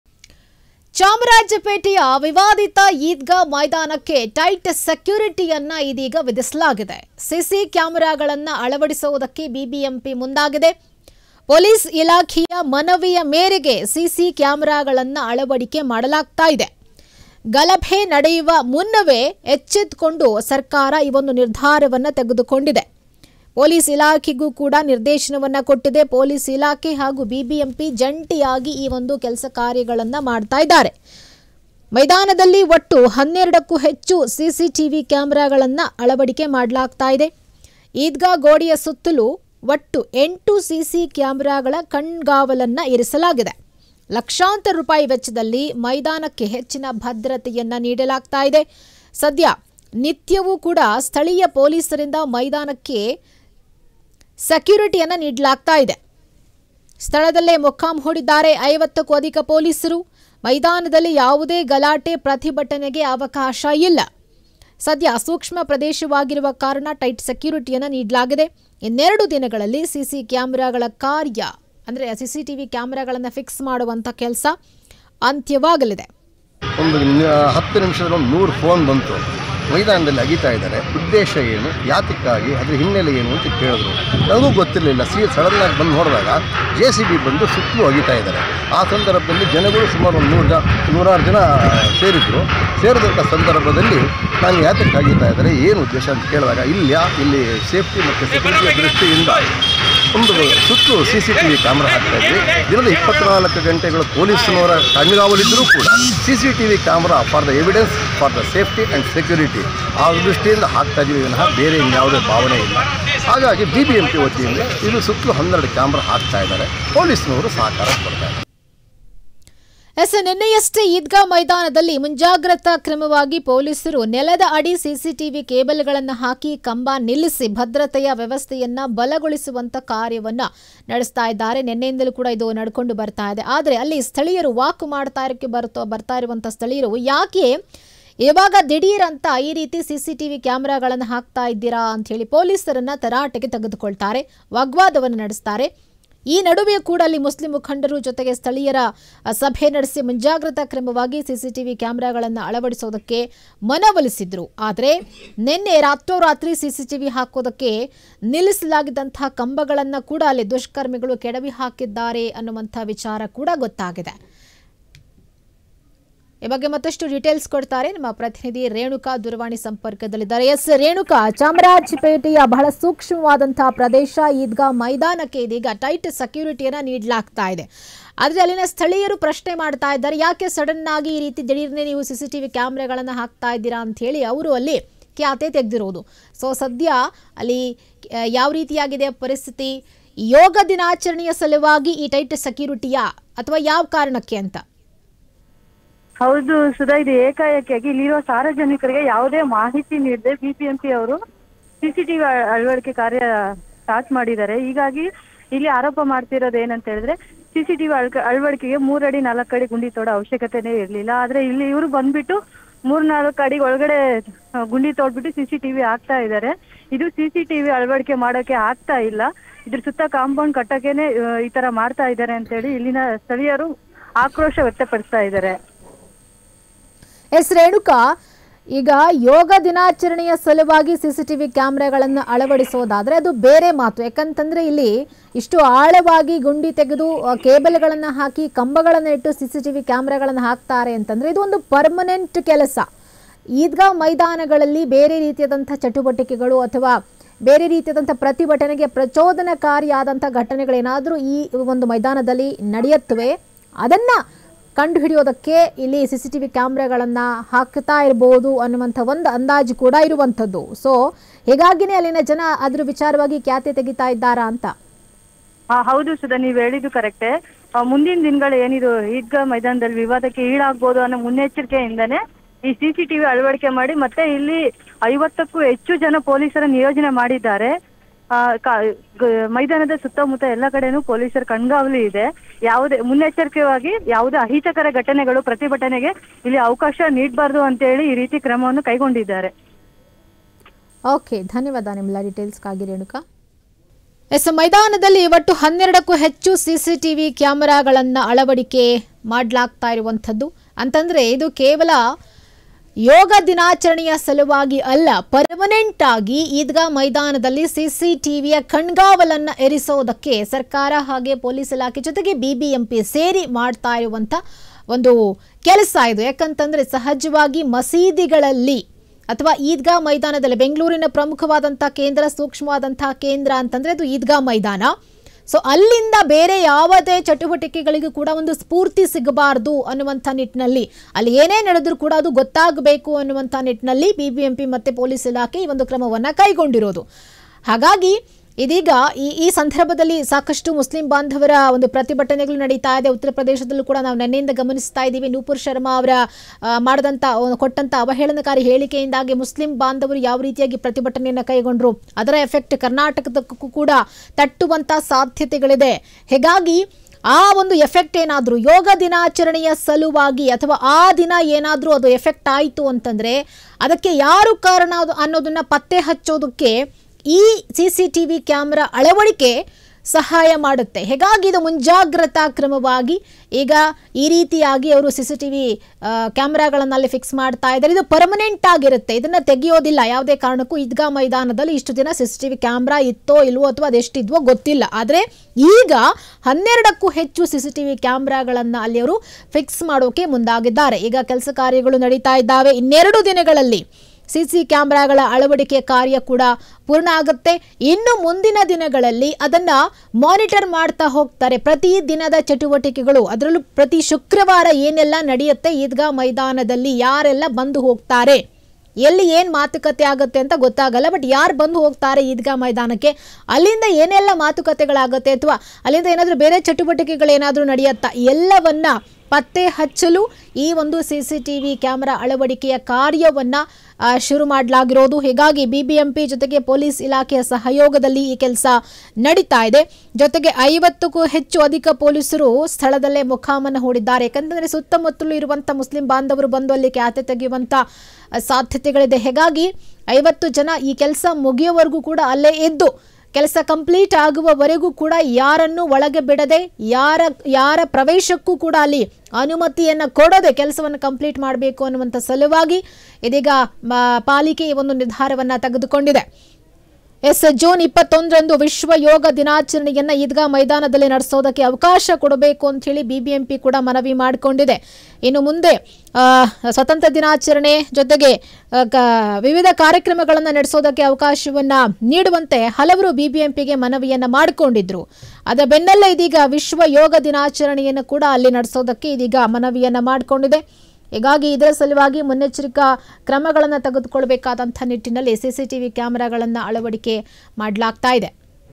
ಚಾಮರಾಜಪೇಟೆಯ ವಿವಾದಿತ ಈದ್ಗಾ ಮೈದಾನಕ್ಕೆ ಟೈಟ್ ಸೆಕ್ಯುರಿಟಿ | Chamarapet | Edga | Bengaluru 1 News
ಸಿಸಿ ಕ್ಯಾಮರಾಗಳನ್ನ ಅಳವಡಿಸಿಲು ಮುಂದಾದ ಬಿಬಿಎಂಪಿ | ಪೊಲೀಸ್ ಇಲಾಖೆಯ ಮನವಿ ಮೇರೆಗೆ ಸಿಸಿ ಕ್ಯಾಮರಾ ಅಳವಡಿಕೆ | ಗಲಭೆ ನಡೆಯುವ ಮುನ್ನವೇ ಎಚ್ಚೆತ್ತು ಈ ನಿರ್ಧಾರ |
ಪೊಲೀಸ್ ಇಲಾಖೆ ಹಾಗೂ ಬಿಬಿಎಂಪಿ ಜಂಟಿಯಾಗಿ ಕಾರ್ಯ|
ಮೈದಾನದಲ್ಲಿ ಒಟ್ಟು 12ಕ್ಕೂ ಹೆಚ್ಚು ಸಿಸಿ ಕ್ಯಾಮರಾಗಳ ಅಳವಡಿಕೆ |
ಈದ್ಗಾ ಗೋಡೆಯ ಸುತ್ತಲೂ ಒಟ್ಟು 8 ಸಿಸಿ ಕ್ಯಾಮರಾಗಳ ಕಣ್ಗಾವಲು |
ಲಕ್ಷಾಂತರ ರೂಪಾಯಿ ವೆಚ್ಚದಲ್ಲಿ ಮೈದಾನಕ್ಕೆ ಹೆಚ್ಚಿನ ಭದ್ರತೆ |
ಸದ್ಯ ನಿತ್ಯ ಸ್ಥಳೀಯ ಪೊಲೀಸರಿಂದ ಮೈದಾನಕ್ಕೆ ಮುಂದುವರಿದ ಸೆಕ್ಯುರಿಟಿ |
ಸ್ಥಳದಲ್ಲೇ ಮೊಕ್ಕಾಂ ಹೂಡಿರಿರುವ 50ಕ್ಕೂ ಅಧಿಕ ಪೊಲೀಸರು |
ಮೈದಾನದಲ್ಲಿ ಯಾವುದೇ ಗಲಾಟೆ, ಪ್ರತಿಭಟನೆಗೆ ಅವಕಾಶ ಇಲ್ಲ |
ಸದ್ಯ ಸೂಕ್ಷ್ಮ ಪ್ರದೇಶವಾಗಿರುವ ಕಾರಣ ಟೈಟ್ ಸೆಕ್ಯುರಿಟಿ |
ಇನ್ನೆರಡು ದಿನಗಳಲ್ಲಿ ದಿನಗಳಲ್ಲಿ ಸಿಸಿ ಕ್ಯಾಮರಾಗಳ ಕಾರ್ಯ ಅಂತ್ಯ.